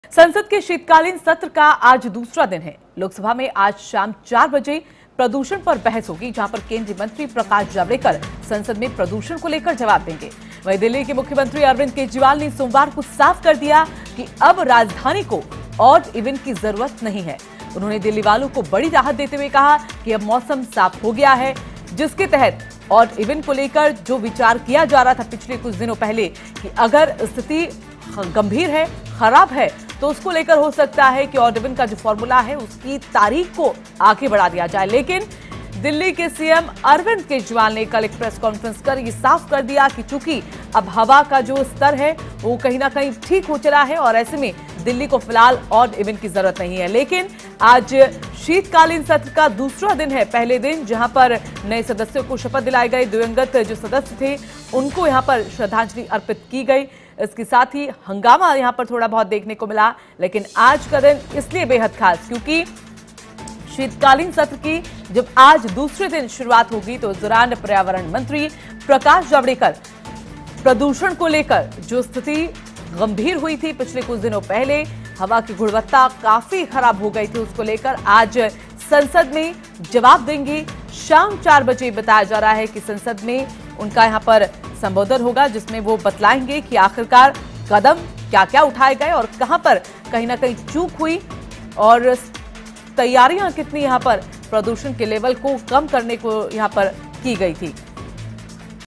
संसद के शीतकालीन सत्र का आज दूसरा दिन
संसद के शीतकालीन सत्र का आज दूसरा दिन है...लोकसभा में आज प्रदूषण पर बहस होगी. केंद्रीय मंत्री प्रकाश जावड़ेकर संसद में प्रदूषण पर जवाब देंगे...